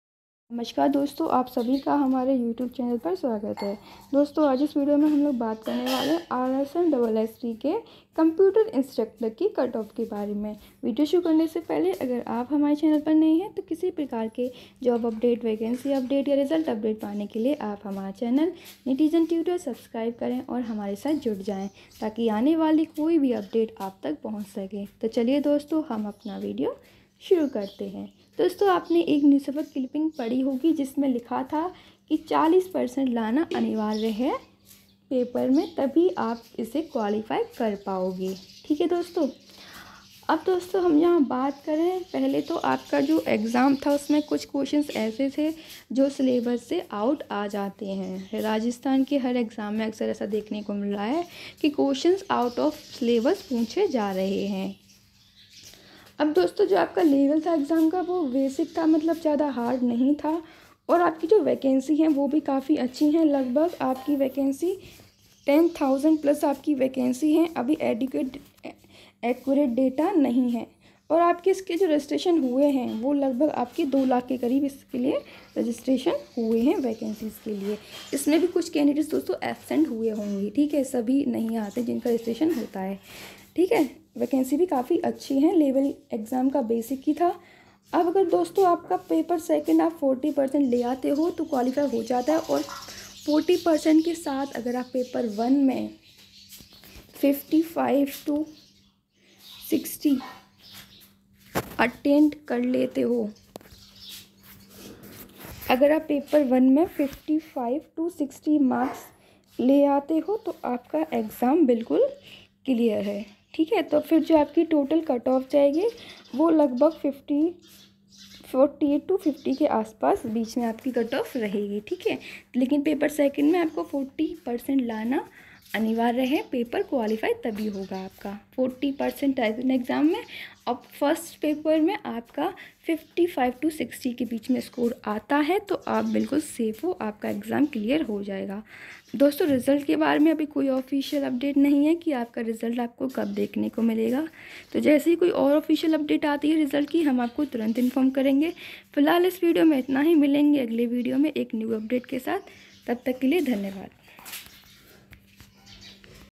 RSMSSB Computer Instructor Cutoff | RSMSSB Computer Instructor Result
About:-
In this video, I have informed about the new update regarding the declaration of result of RSMSSB.

post rajasthan rsmssb computer instructor recruitment online form 2022,
rsmssb basic & sr computer instructor recruitment online form 2022,